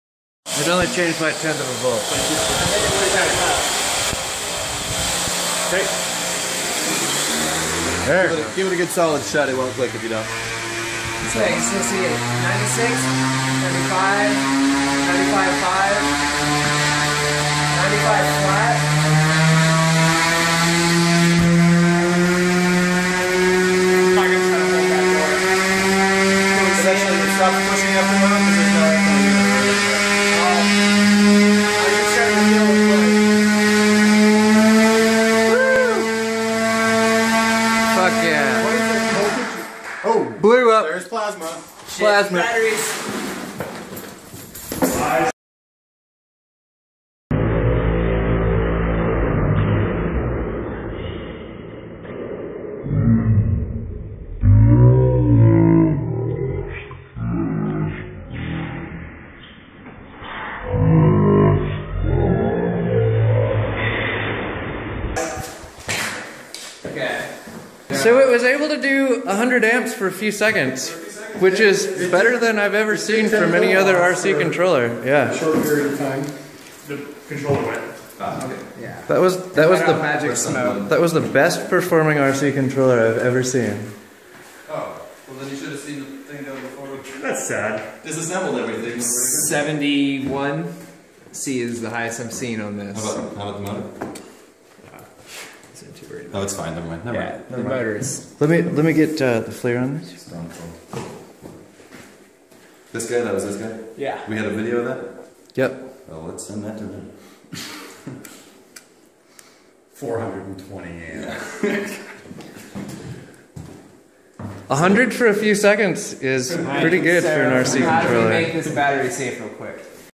Plasma emission at 100amps. Magic smoke refill needed.
Testing the APS 420amp RC controller on a dyno.  This was the 2nd stage power test to move up to 100amps at 90vdc.  It previously ran very well at both 30amps and 50amps.  Not bad for an RC controller, I was impressed.